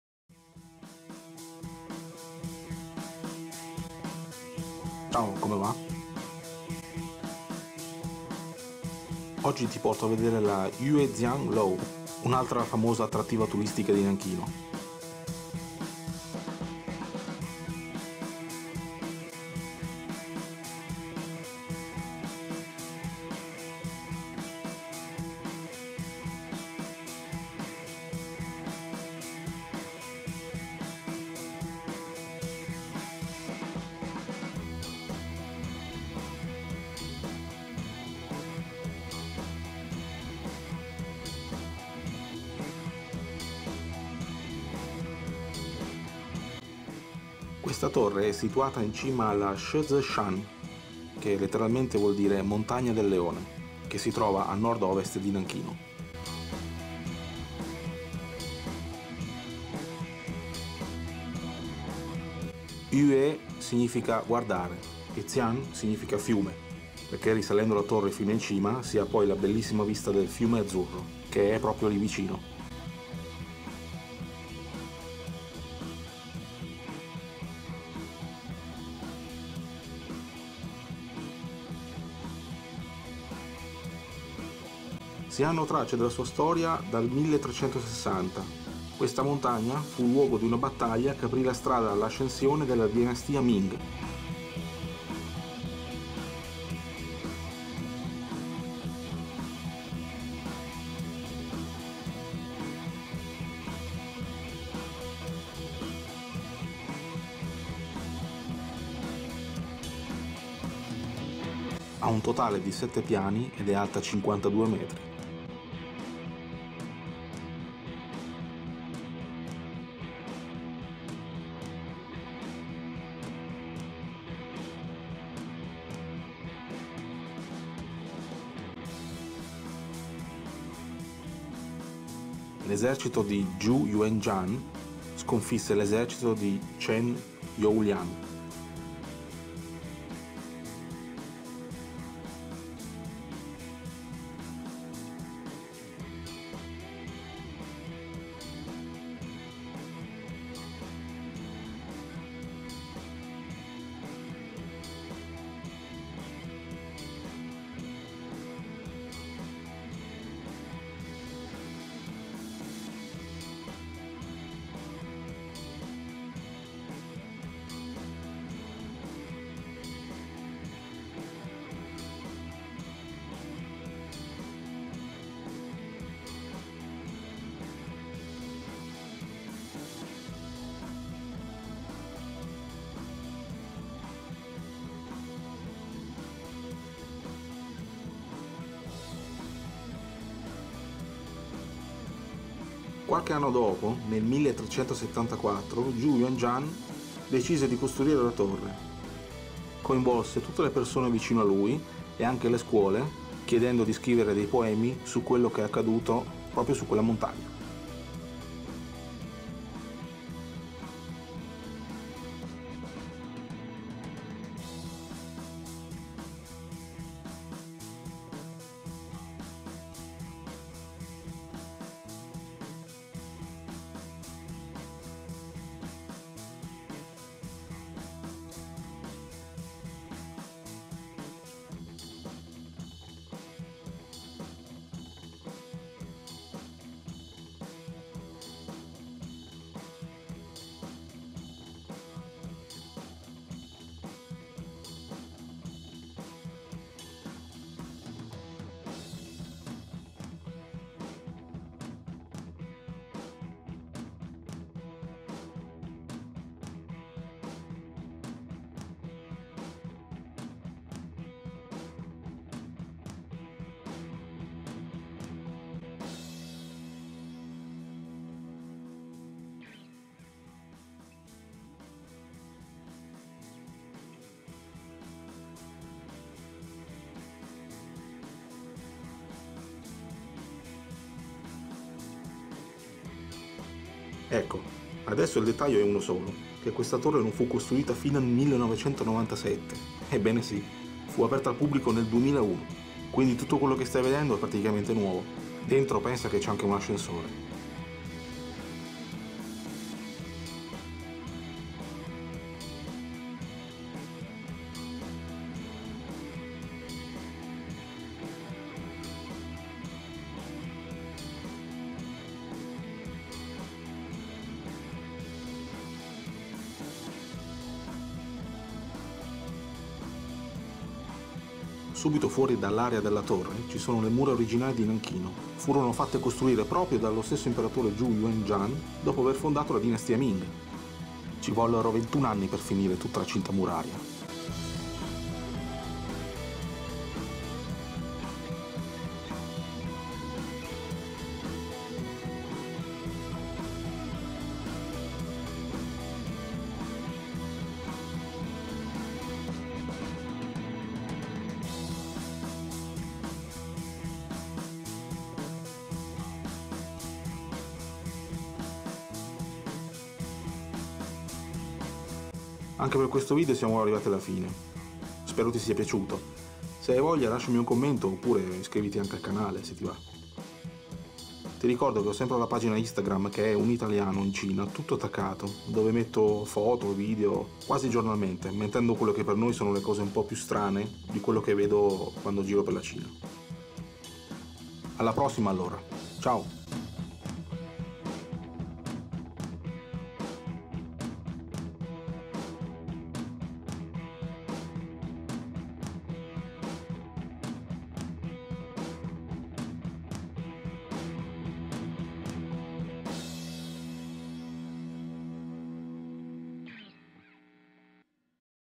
NANJING - 南京 - Yuejiang Tower / Un Italiano in CINA
Ti mostro un altra grande attrattiva turistica di Nanchino. la Yuejiang Tower